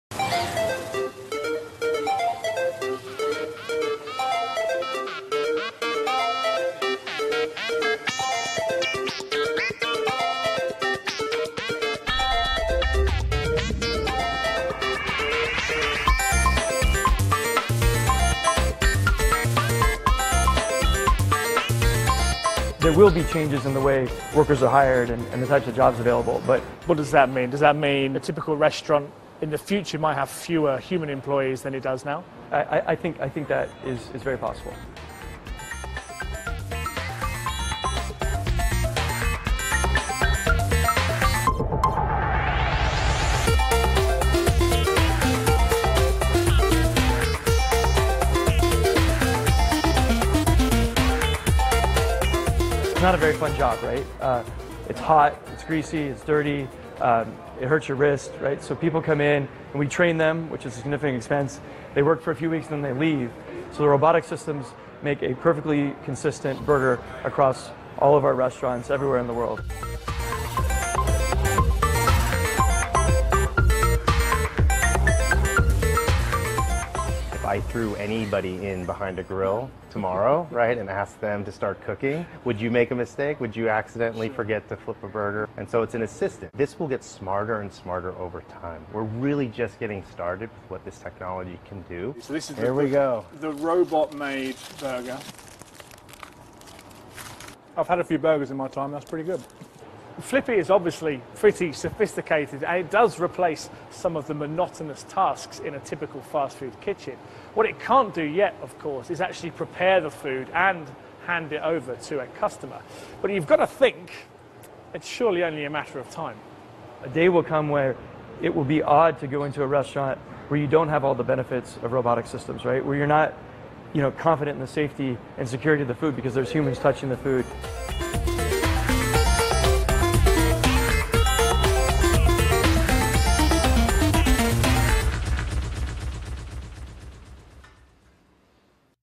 Flippy - A Burger-Flipping Robot
California Restaurant Begins Using "Flippy", A Burger-Flipping Robot Alternative To Wage-Earning Workers!

A burger-flipping robot that doesn’t require a paycheck or benefits — and can grill 150 burgers per hour — is now a cook at CaliBurger.